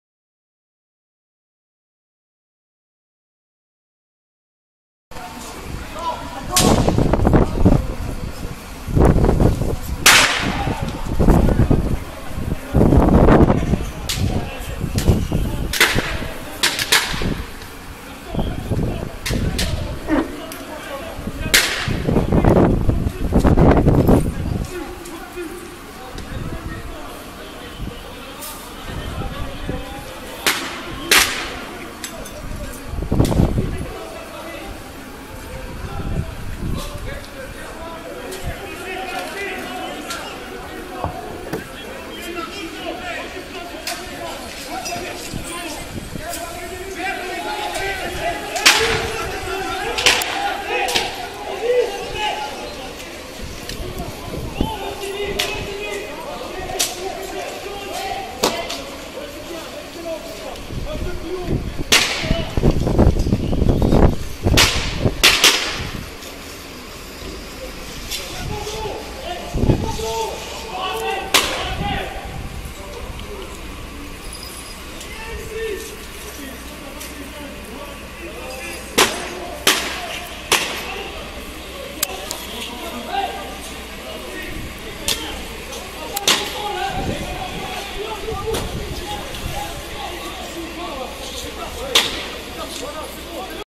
Assault rue de la colline, Verviers. 15.01.15' (Belgium RAID) -Fix Brightness 1080p
Not a news site but I just fixed the video up a little bit with a tad more brightness since no one bothered I have.

To everybody! this vidéo is total free, make a lot of money with it, dudes! But please, tell this to the world: We don't want civil war anyway!
Say it, it's my condition. Thank you.

▶ & Google+